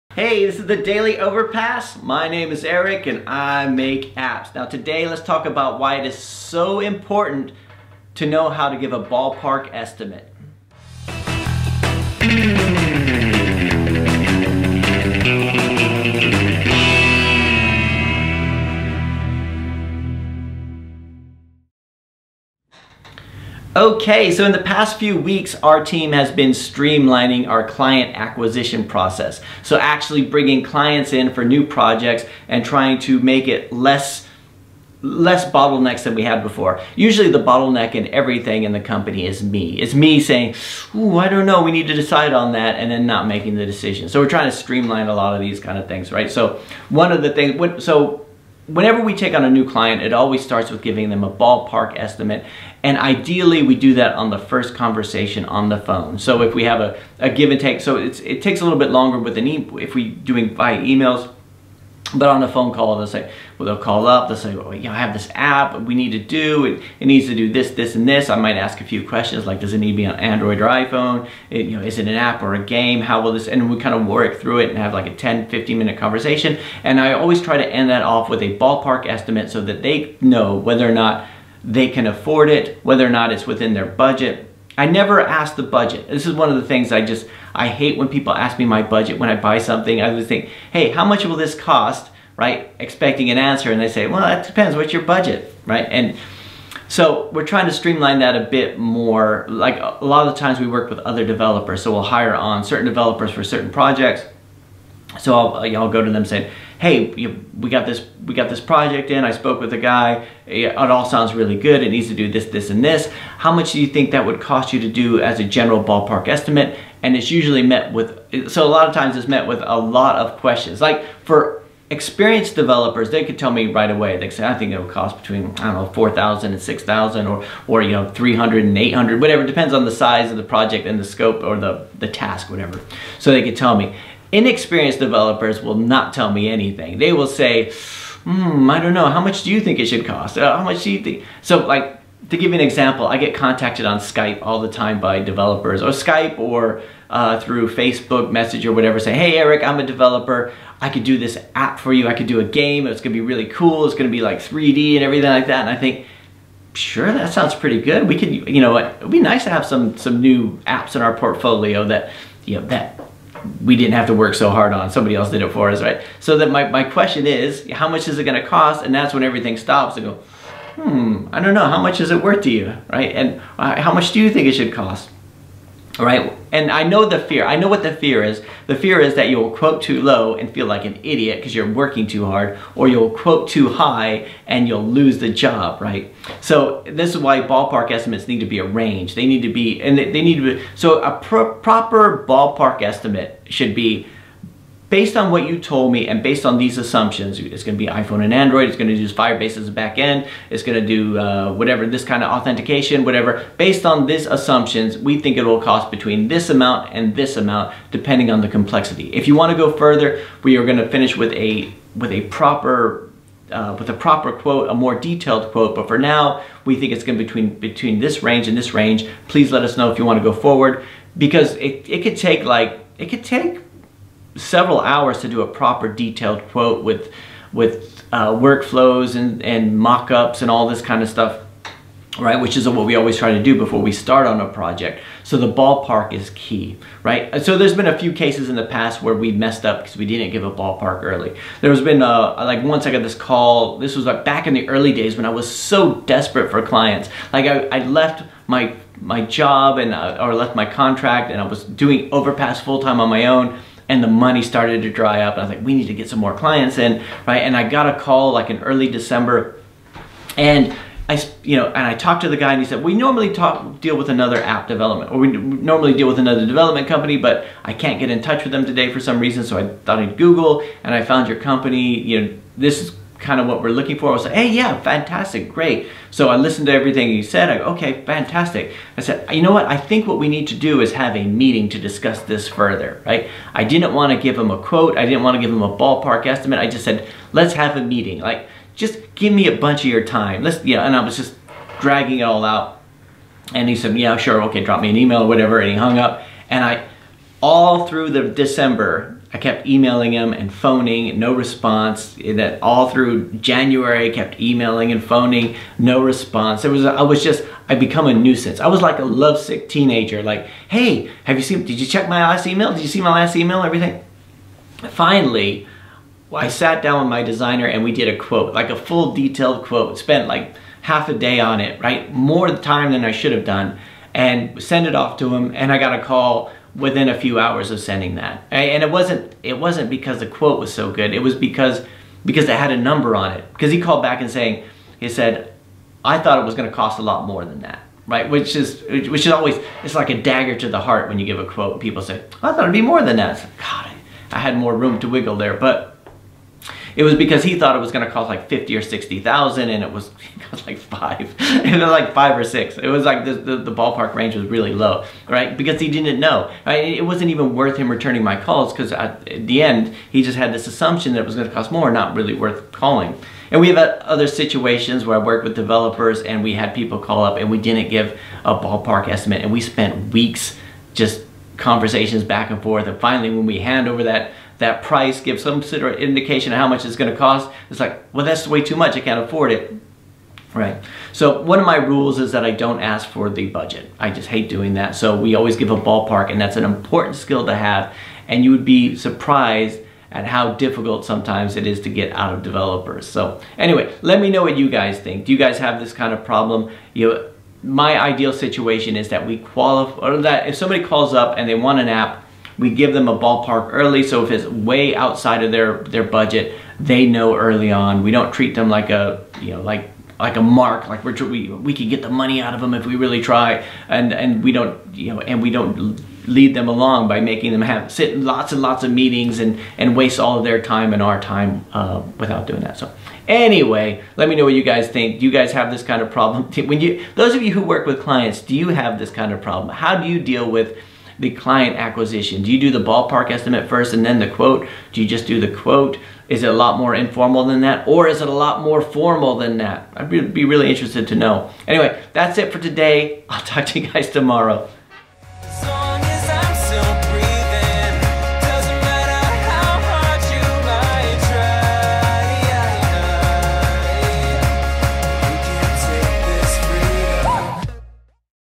The Importance of Knowing How to Give a Ballpark Estimate
The Importance of Knowing How to Give a Ballpark Estimate (The Daily Overpass #311)

Today, I talked about the importance of knowing how to give a ballpark estimate. I talked about how we are streamlining our system and our standard operating procedures, especially with our client projects.

So...

Or...